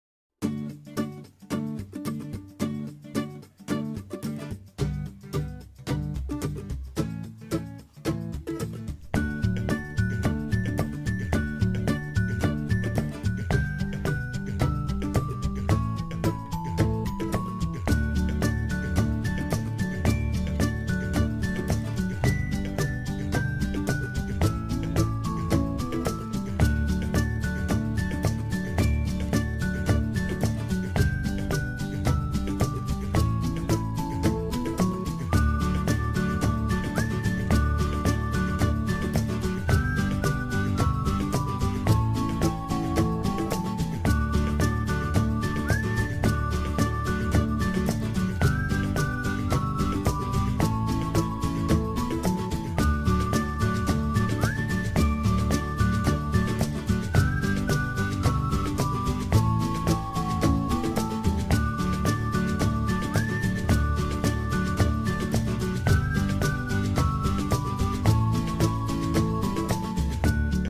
M.k.Plastic Wire Works (1 Roll - Flower Design Basket)
Wires - 30 (6 Feet)
Base -5 lines (4 & 5 Corner Turn)
Handle -6 (3+3)-5 1/2 Feet
For Flower Design - 1 Feet
Knot - Basic & Jasmine knot